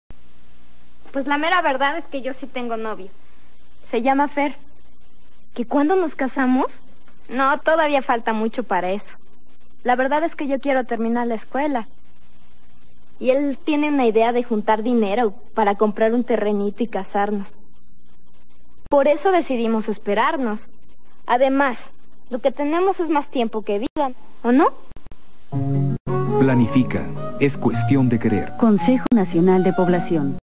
Comerciales mexicanos: CONAPO 1995 3
Esta es la primera etapa de la campaña de Planifica es cuestión de querer aunque el logo del conapo se haya modificado este año se le puso abajo el significado de esta institución